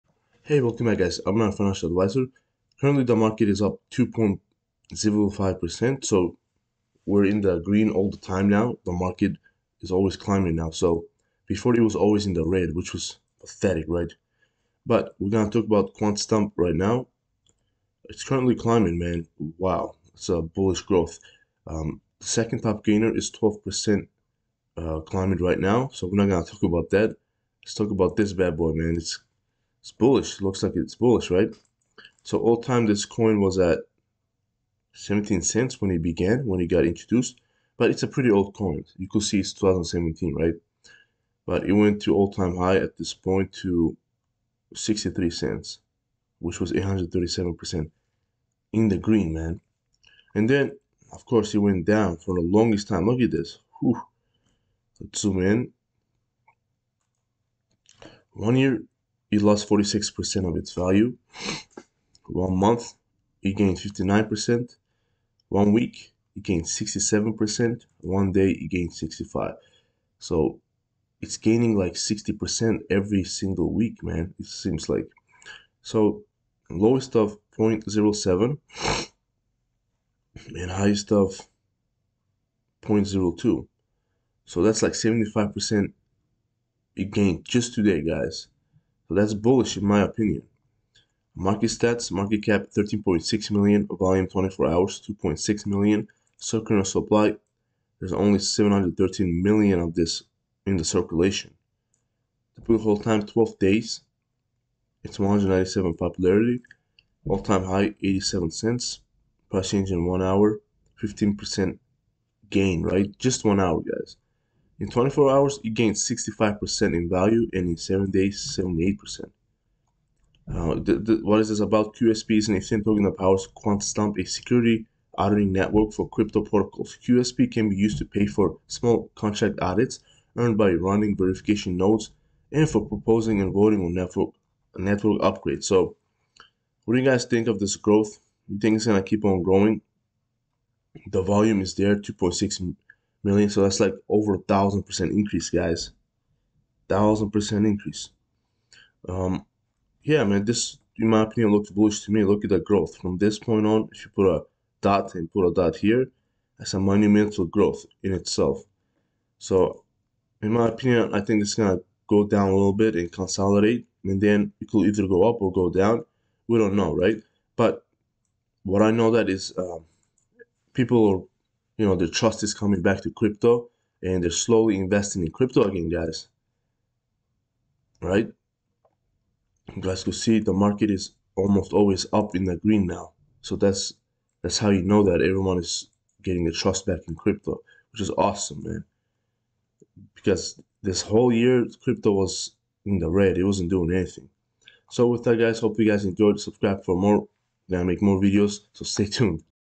Quantstamp Crypto is Pumping up right now with Bullish Growth! Over 70% increase in value! Insane!
Insane Growth Happening on this crypto right now! Bullish!